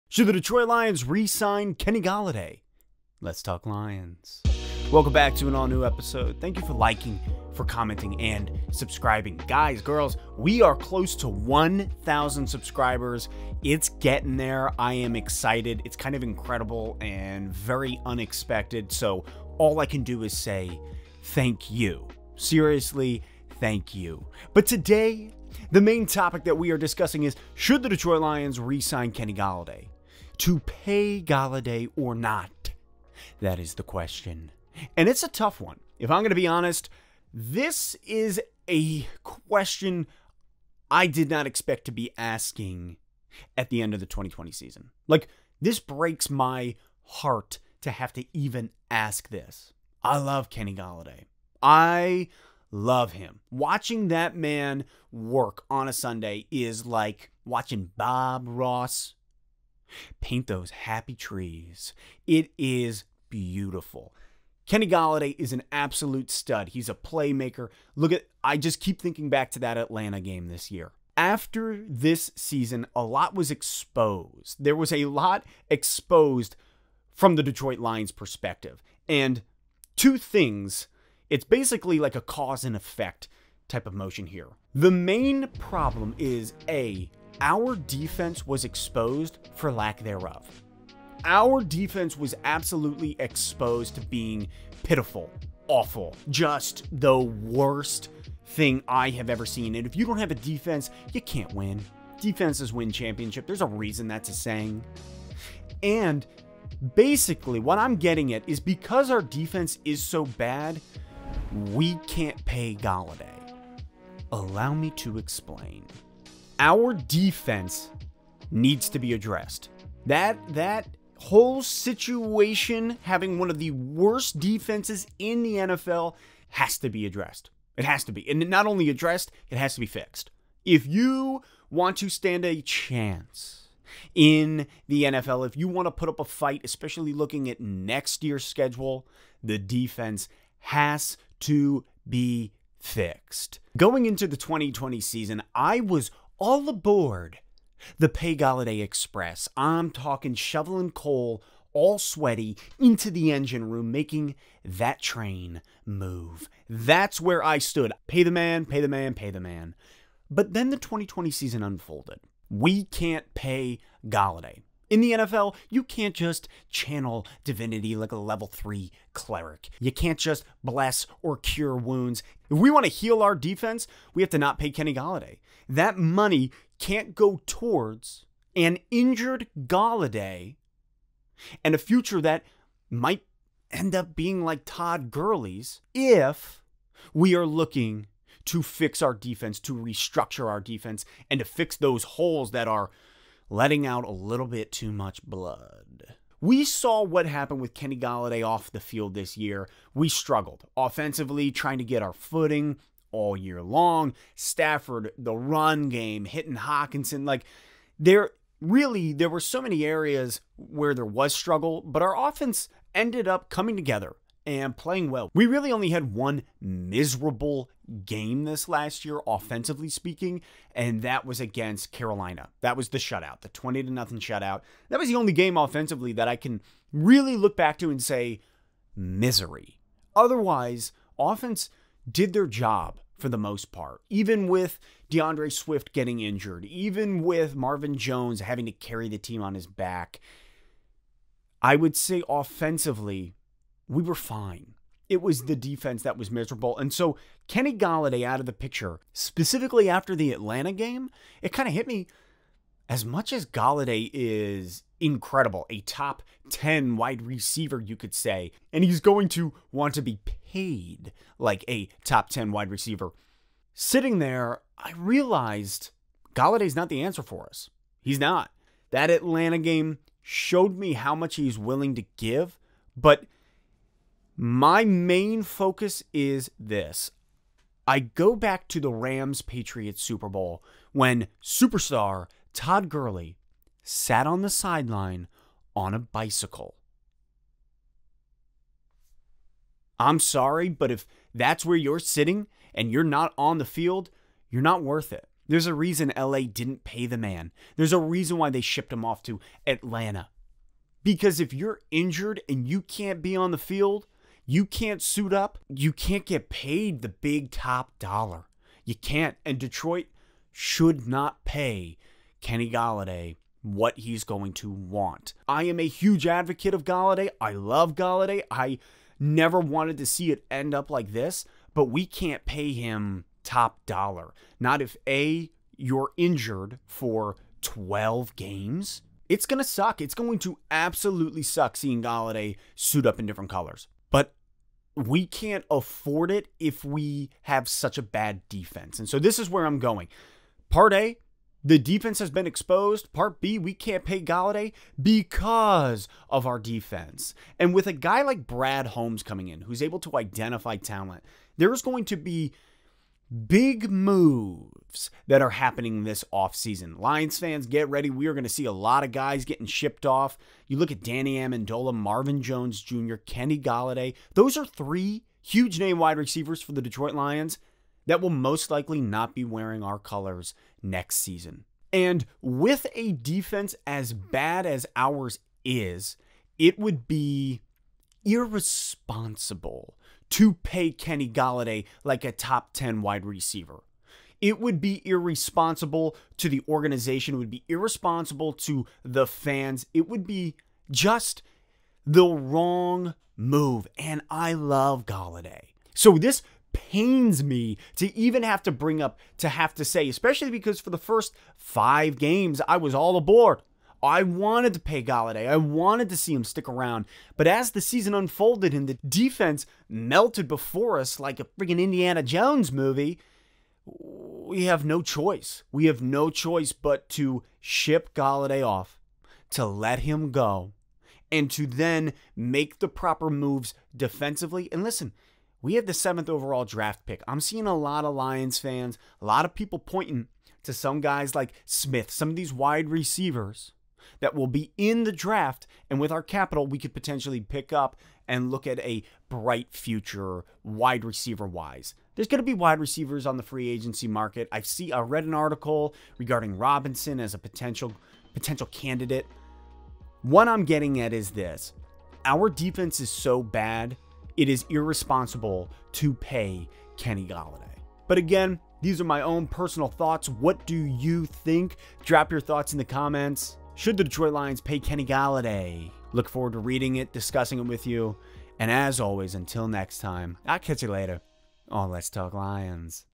Should the Detroit Lions PAY Kenny Golladay?
A huge question for the Detroit Lions this offseason is should they resign Kenny Golladay? Interested in what gear I use?